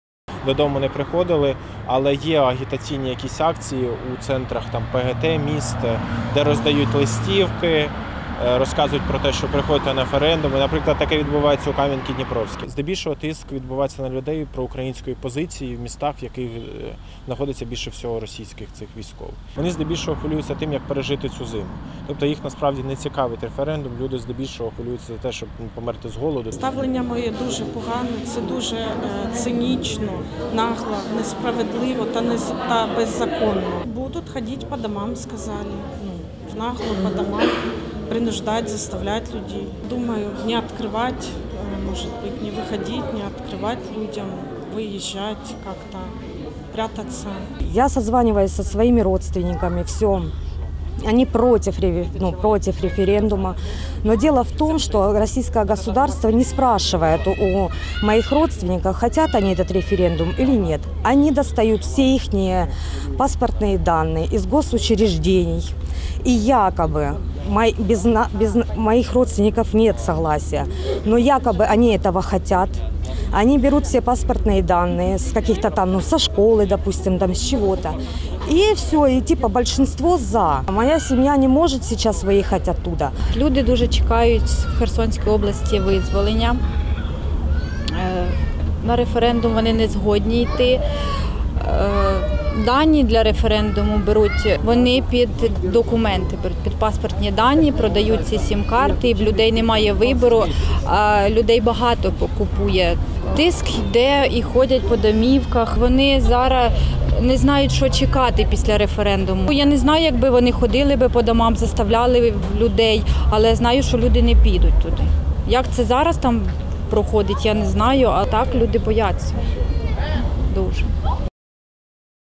Вимушені переселенці, рідні яких в окупації, розповіли, що думають про "псевдореферендум" | Новини
Що думають переселенці про псевдореферендум, який відсьогодні відбувається на територіях їх колись постійного місця проживання, як на це реагують їхні рідні та друзі, котрі залишились на окупованих територіях, дізнавалися журналісти Суспільного Запоріжжя.